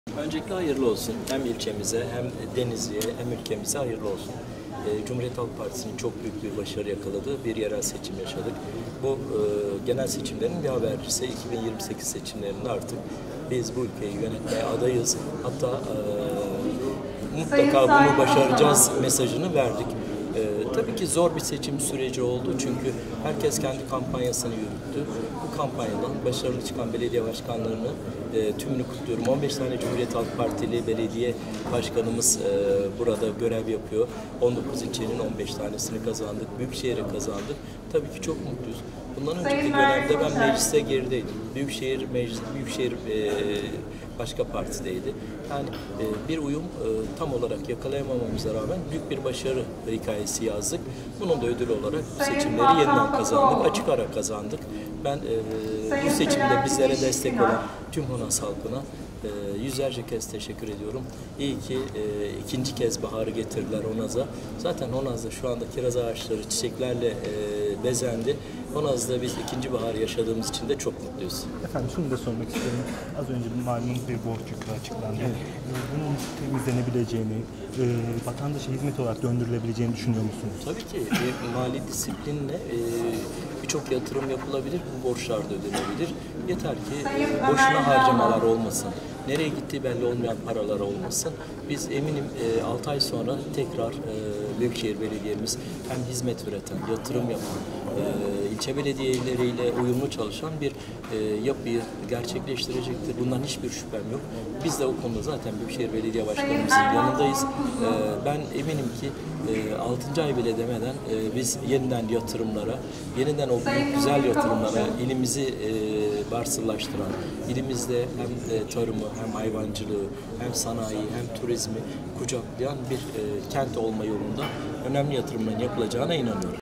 BAŞKAN KEPENEK’TEN İKTİDAR MESAJI
Honaz Belediye Başkanı Yüksel Kepenek, CHP’nin Denizli’de 15 ilçe ve Büyükşehir’i kazanmasının ardından iktidar mesajı verdi. Kepenek, Büyükşehir’in 11 milyar TL’lik borcunun temizlenmesi için ise süre verdi.